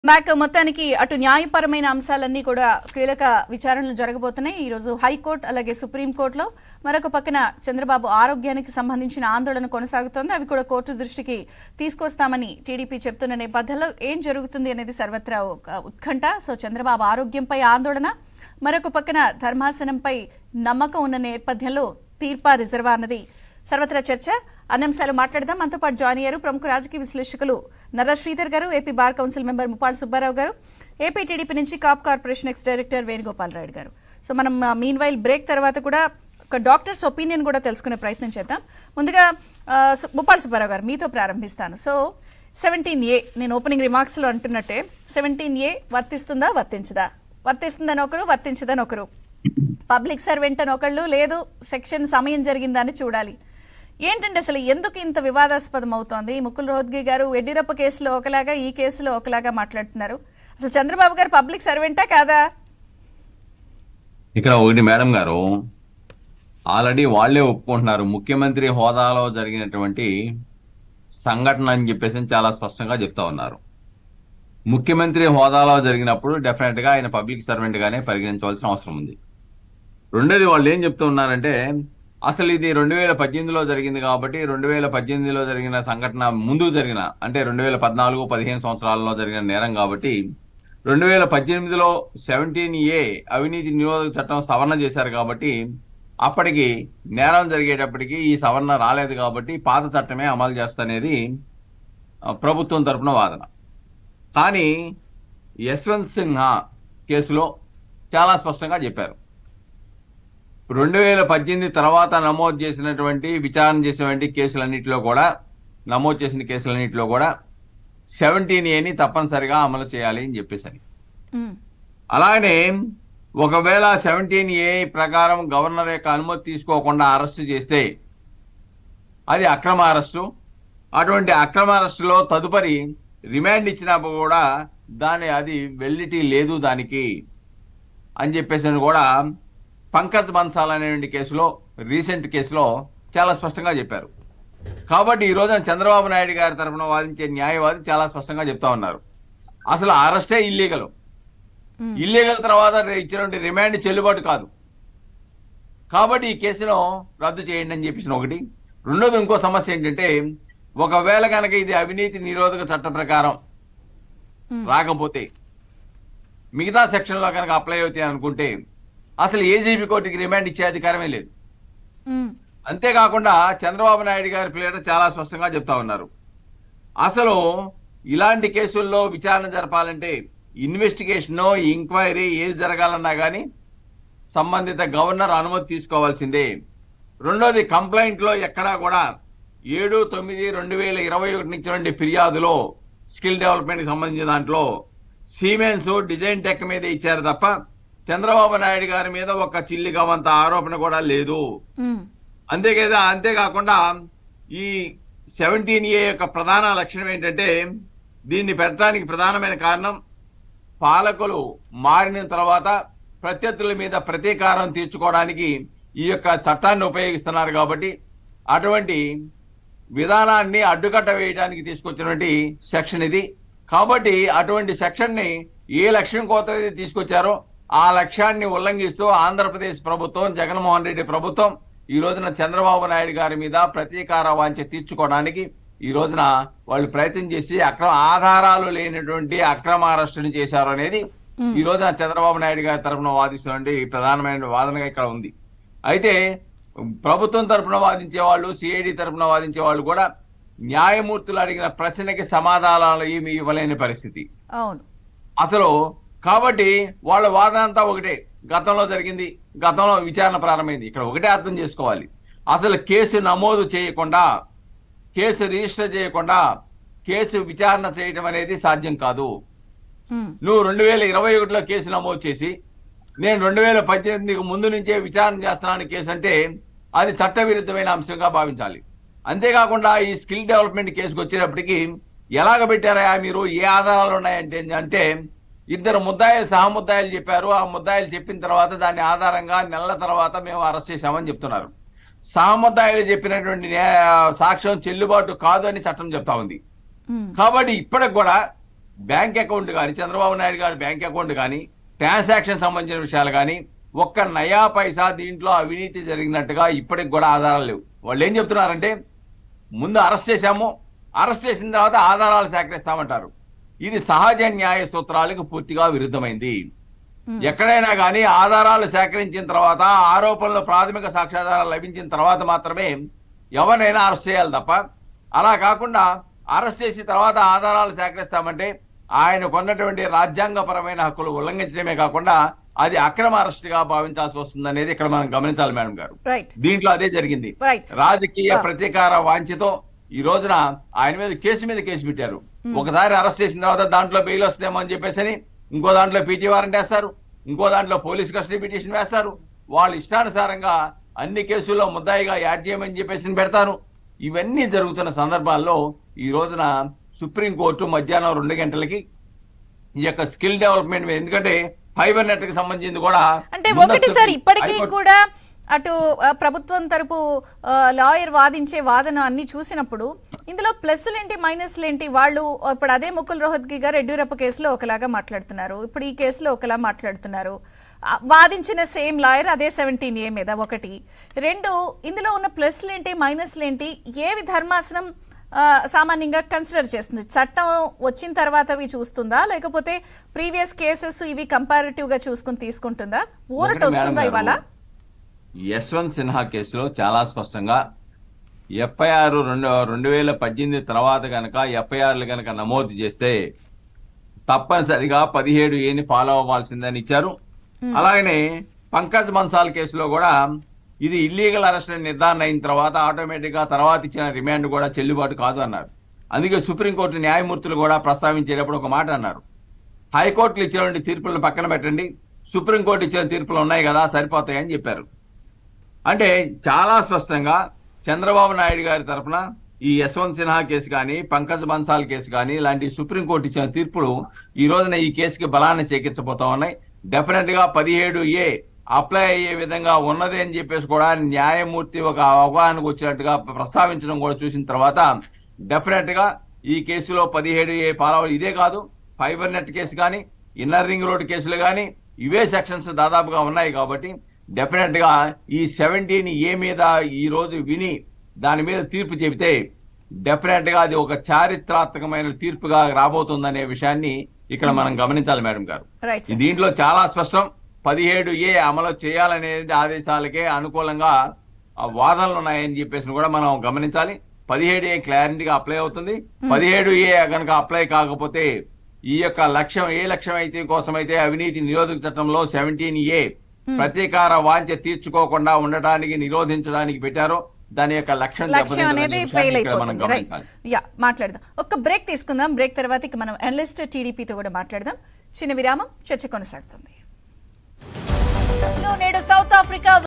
Muppalla Subba Rao : హద్దులు దాటేసిన అధికార విచ్చలవిడితనం !.. తప్పు చేశావ్ జగన్ | Chandrababu | ABN
🔴LIVE : బాబు కేసుల్లో కీలకం.. అనారోగ్యంపై ఆందోళన, ధర్మంపై భరోసా తీర్పా.. రిజర్వా? | Chandrababu |ABN